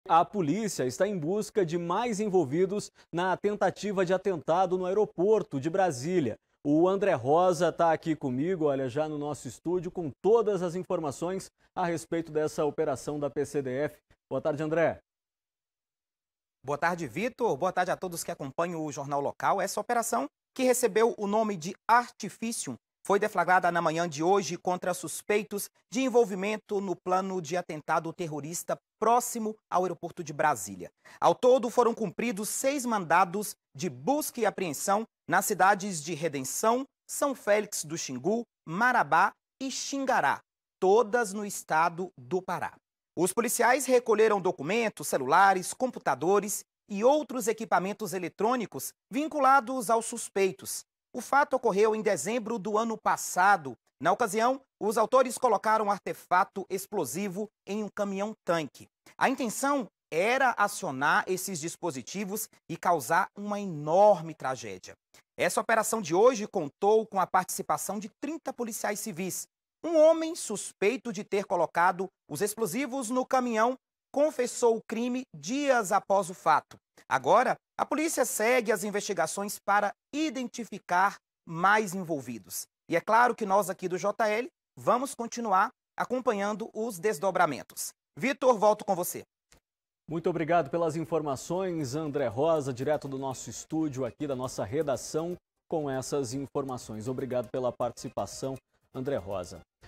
JL - Investigação sobre plano de atentado no aeroporto
14/04 - PCDF recolhe documentos de suspeitos em tentativa de ataque.